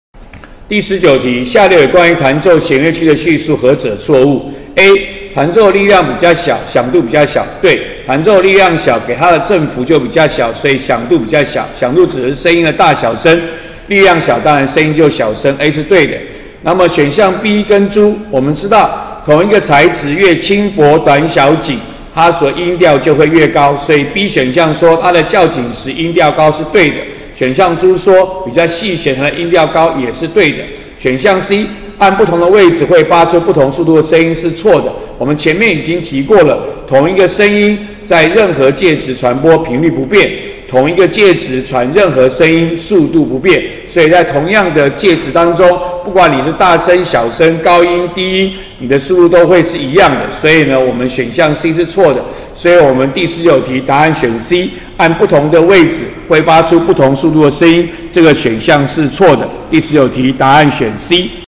103學年上8年級理化二段第19題
103學年上學期八年級理化第二次段考第19題。問弦樂器彈奏力量與響度、鬆緊、粗細與音調、及彈撥位置與聲速的關係。